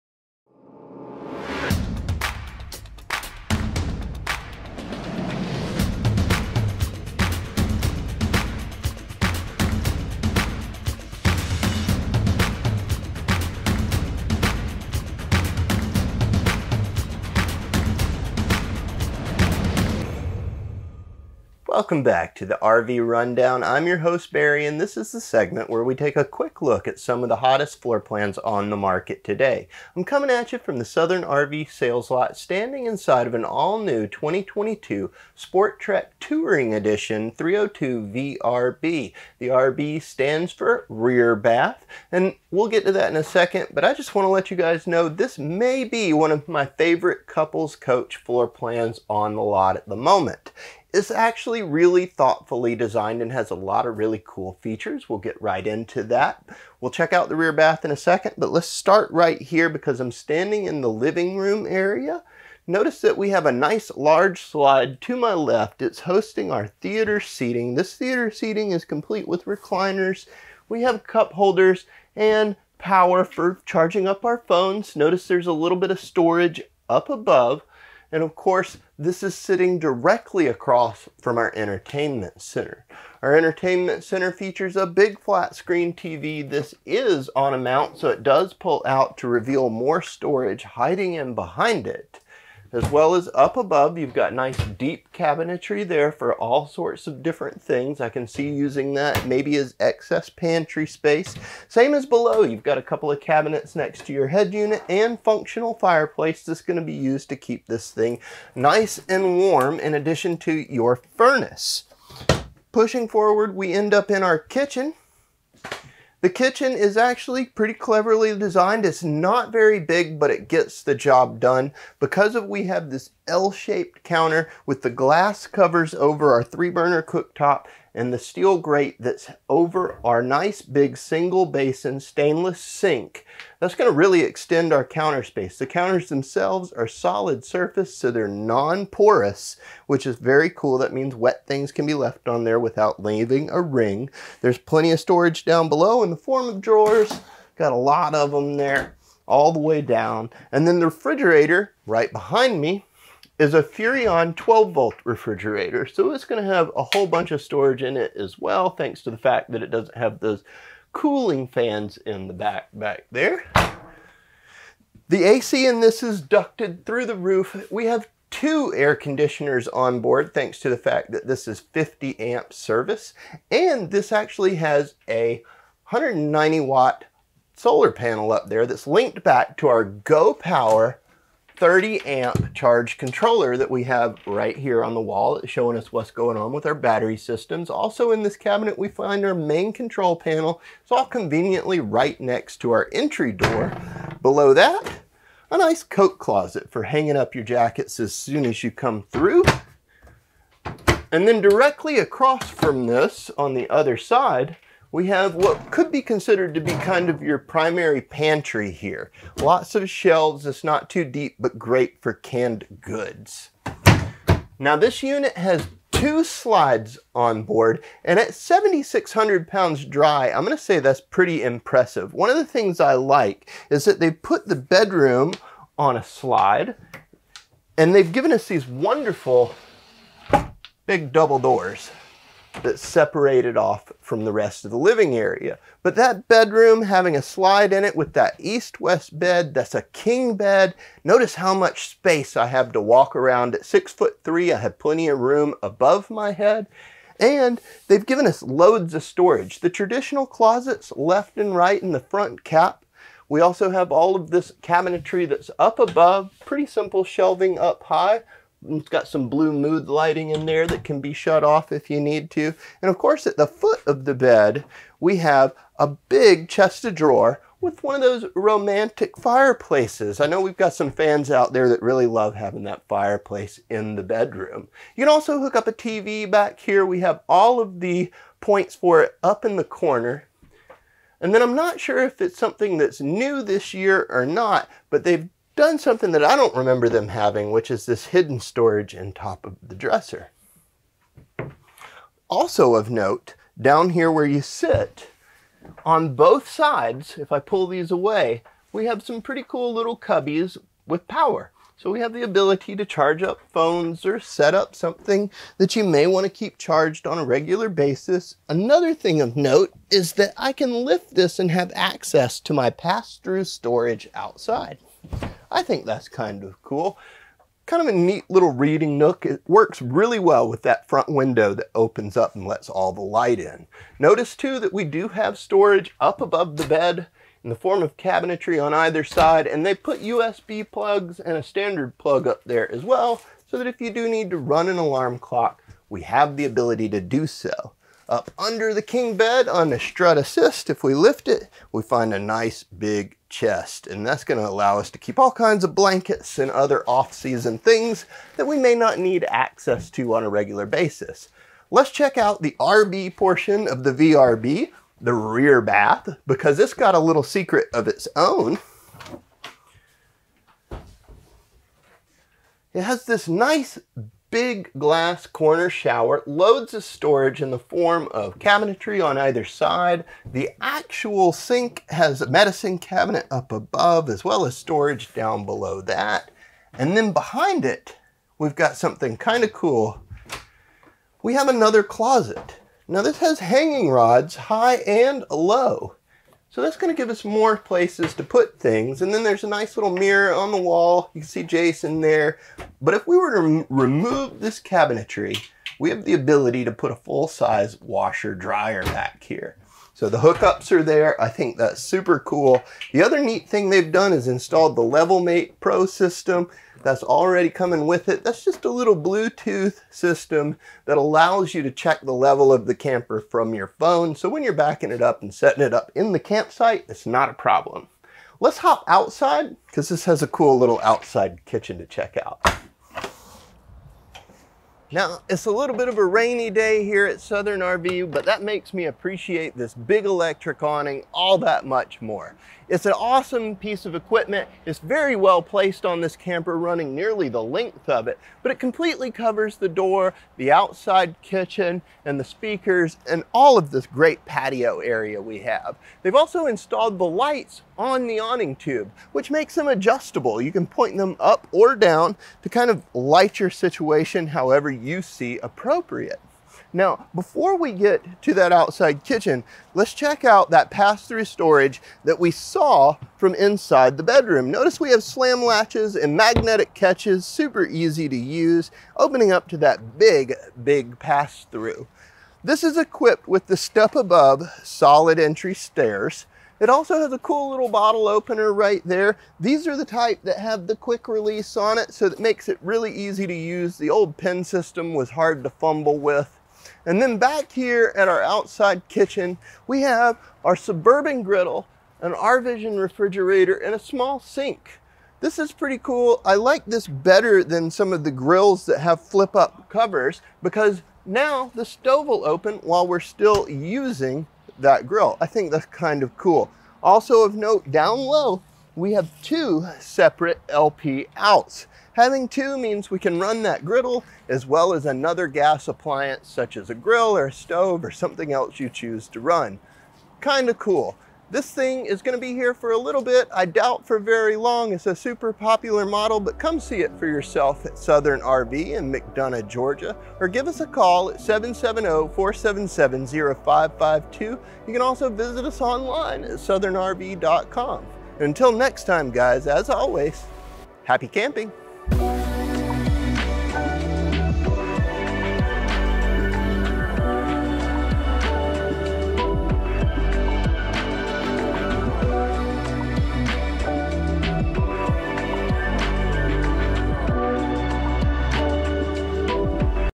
RV Rundown | 2022 Venture RV SportTrek Touring 302VRB Couples Travel Trailer Camper | Southern RV
Stock Number:

Join us for this installment of the RV Rundown where we take a look at the 2022 Venture RV SportTrek 302VRB! This is hands down the best couples coach we have seen! Between the east west king bed and laundry room, this camper is a must see!

324 Industrial Blvd.

". Until next time, happy camping!"